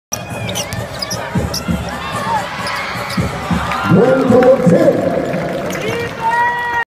Glen Khobuntin spins and scores a bank shot
NLEX rookie Glen Khobuntin twirls then shoots over a defender in their PBA Commissioner's Cup elims game against Ginebra on Feb.12, 2016